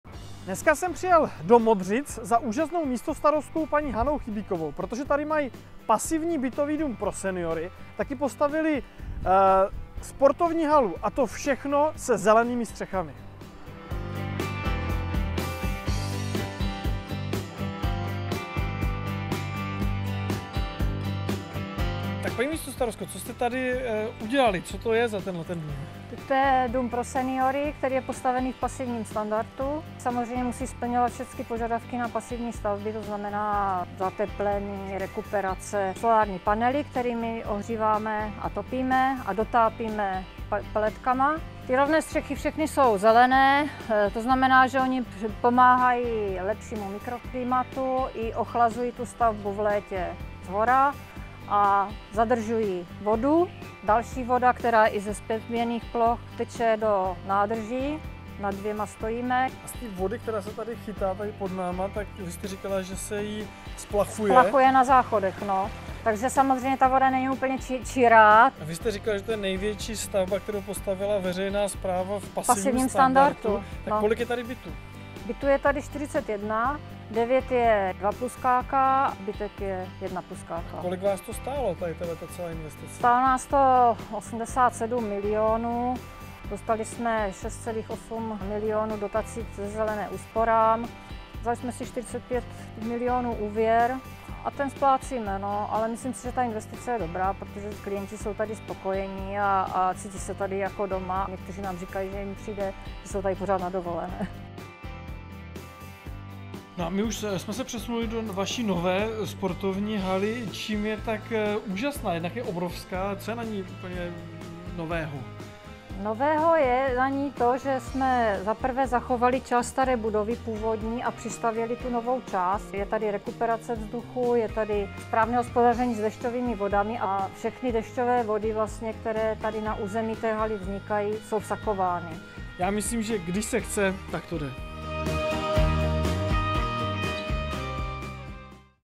Když se chce, tak to jde v Modřicích.
Skvělé pasivní domy se zelenými střechami, fotovoltaikou a využíváním dešťové vody v Modřicích. Když se chce, tak to jde.